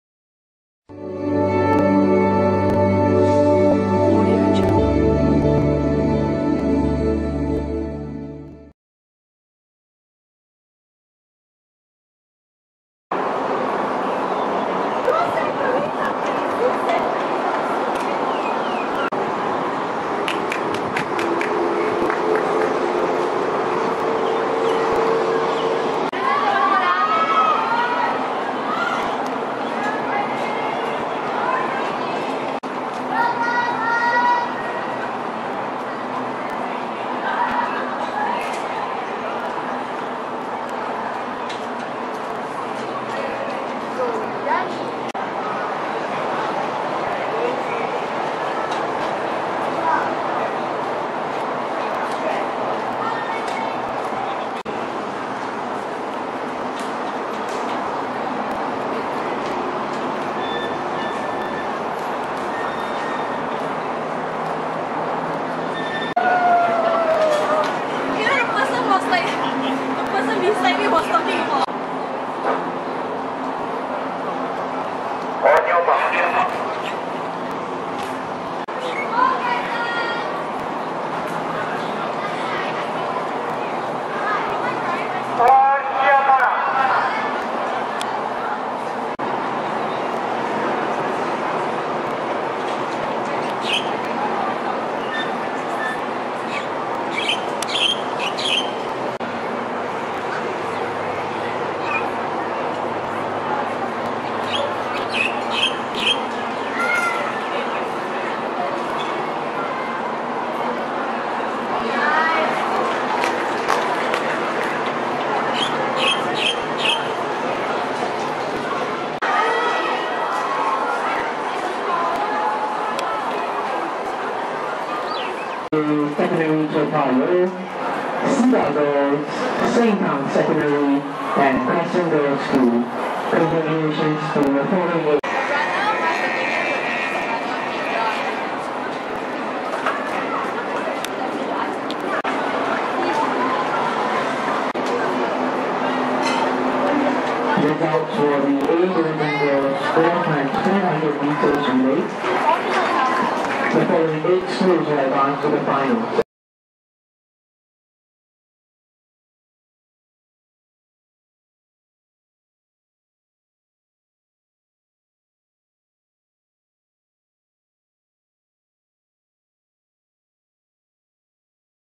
High Jump Girls 'A' Final - National Schools 2012
Dates :

28-30 Mar 2012 National Schools T&F C'ships (AM & PM sessions)
2-4 Apr 2012 National Schools T&F C'ships (AM & PM sessions)
5 Apr 2012 (Thu) National Schools T&F C'ships (AM sessions)
10 Apr 2012 (Tue) National Schools T&F C'ships -- Finals

Not all events/attempts were recorded.